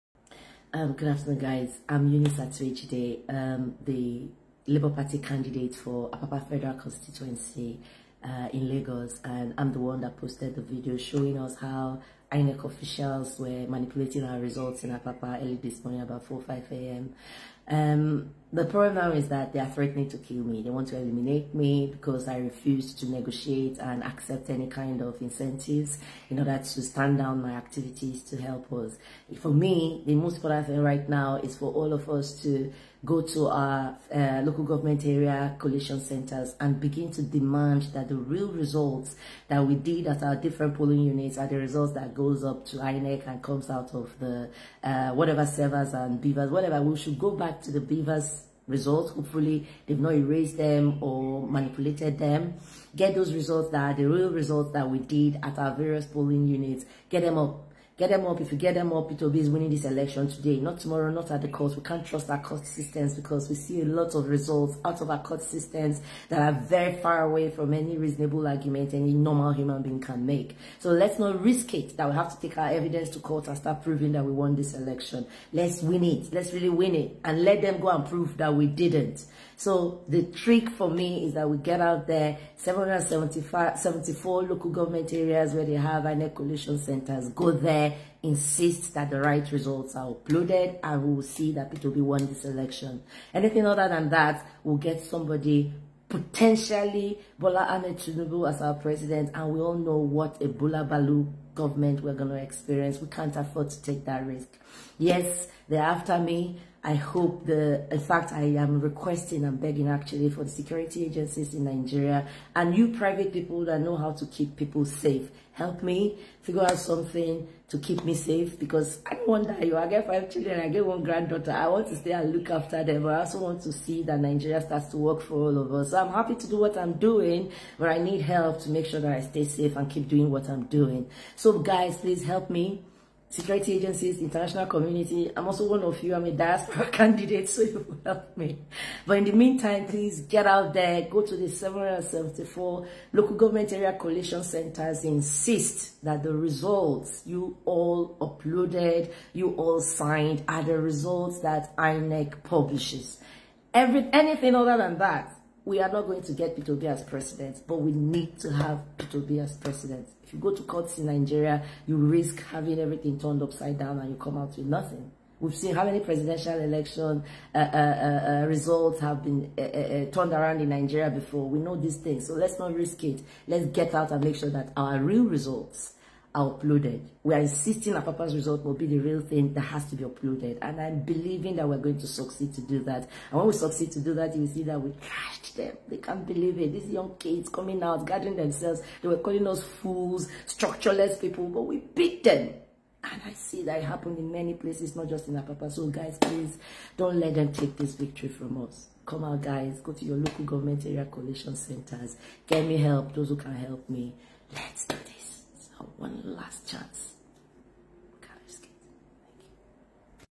Grandmother alleges plan to eliminate her & rig Tinubu into power by eliminating Peter Obi's votes.
Titled: Alleged manipulation of results by INEC staff in Apapa, Lagos for a godfather